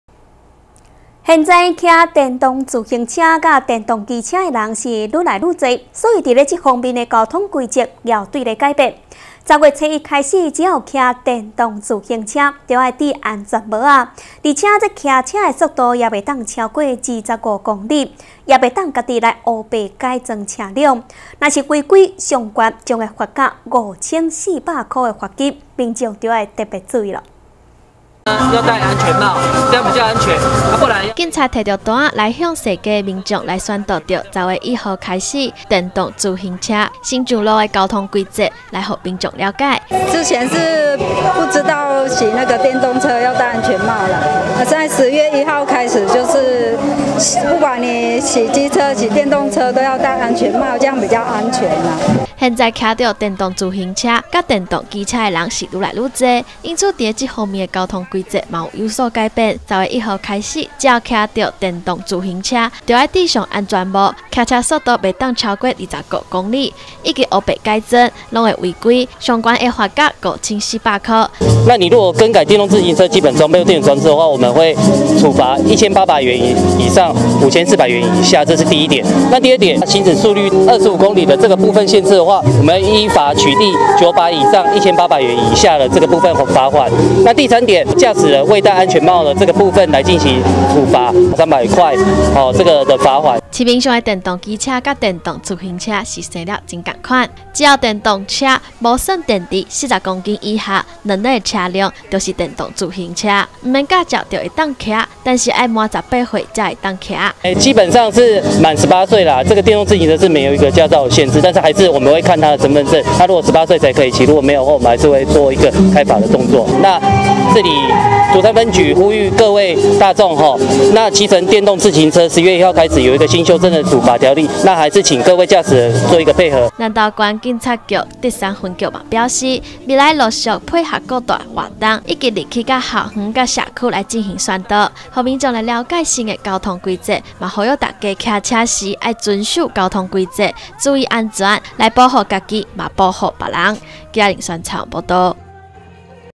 南投新聞 騎乘電動自行車 10月1日起新制上路要注意!
現在騎電動自行車與電動機車的人，越來越多，因此在交通規則上頭，也有所改變，10月1號起，只要騎行電動自行車，要戴上安全帽，騎車速度不能超過25公里，以及不依規定改裝車輛，都進行處罰，罰金最高至五千四百元。

〈以上新聞可從Top NEWS APP 收視〉